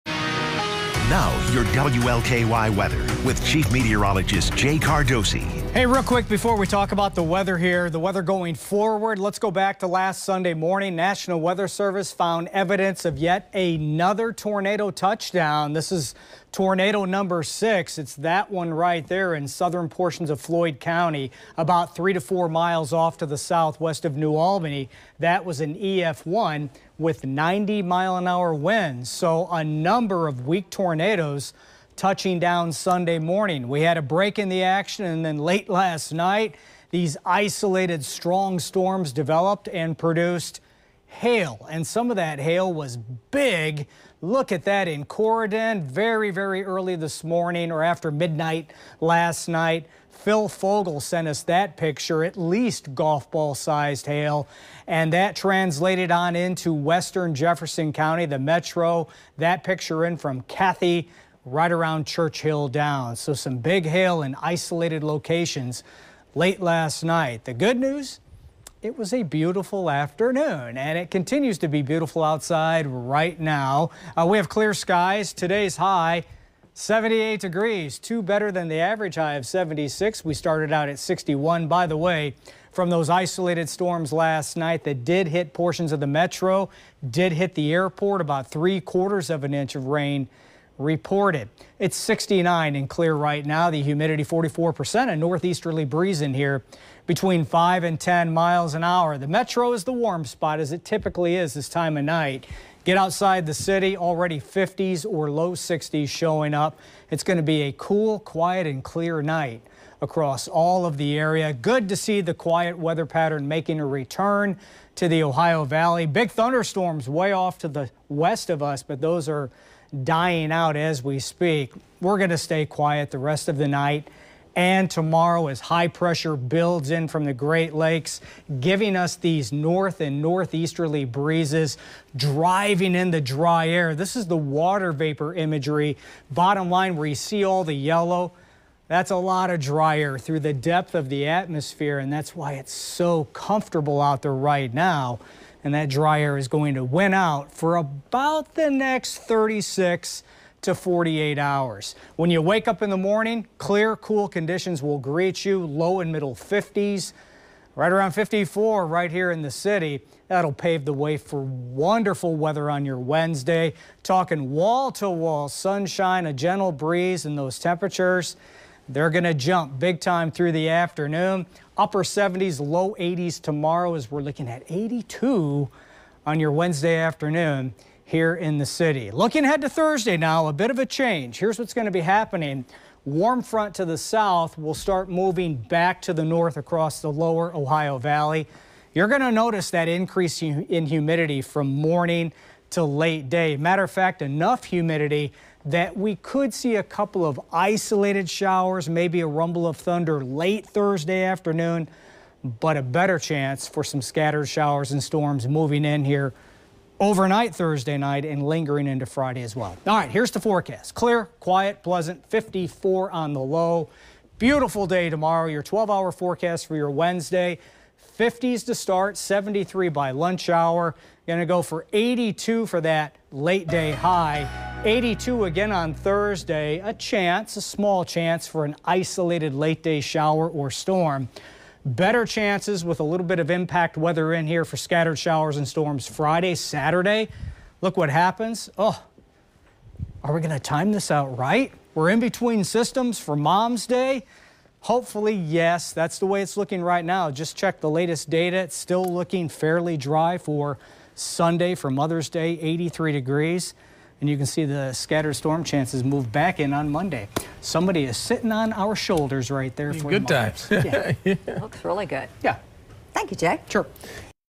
Beautiful weather for Wednesday
Chief meteorologist Jay Cardosi talsk about beautiful Wednesday weather and a return to rain chances late in the work week.